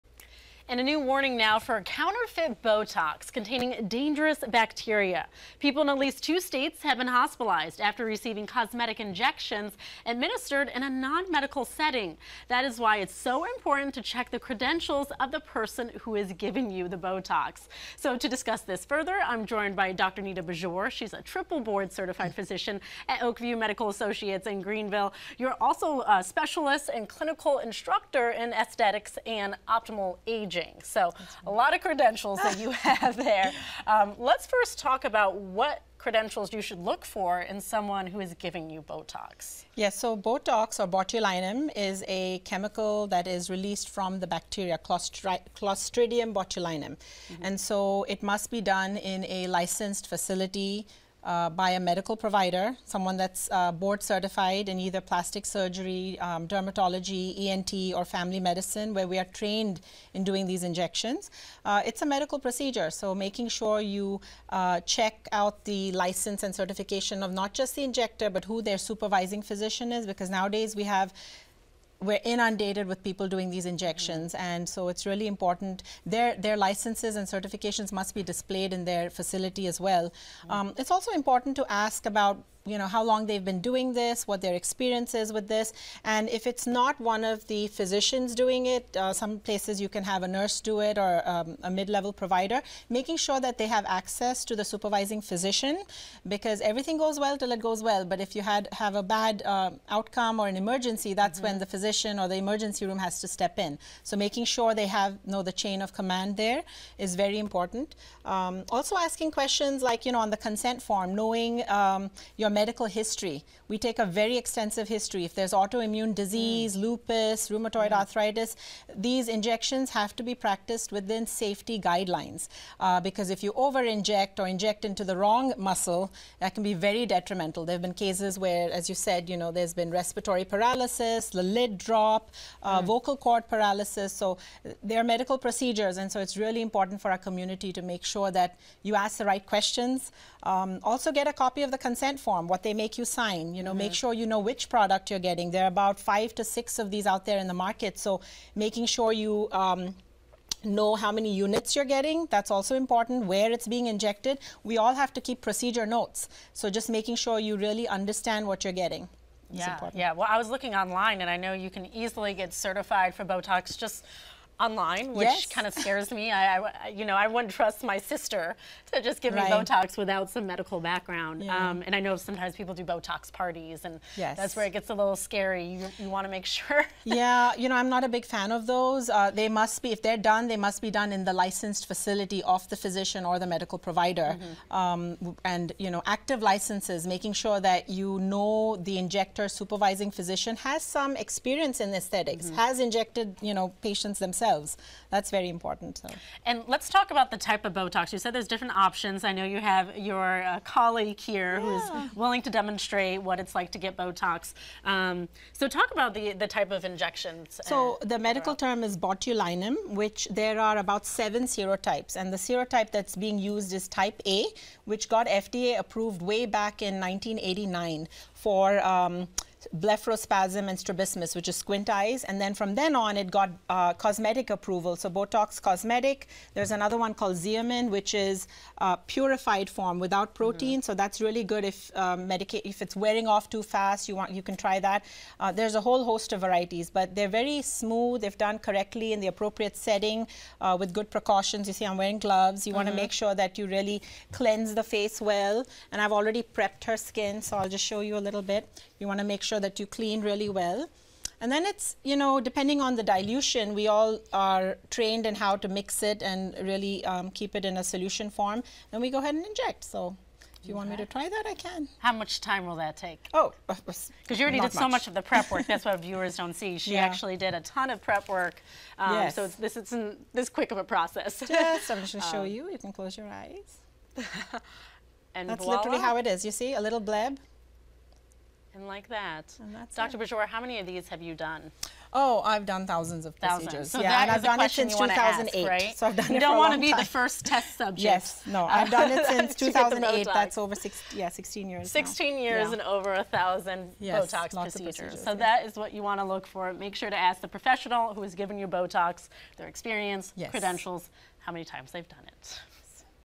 How to spot counterfeit botox before it's administered
Dr. Nita Bijoor from Oakview Medical Associates  joins us to discuss the dangers of counterfeit botox and ways you can spot it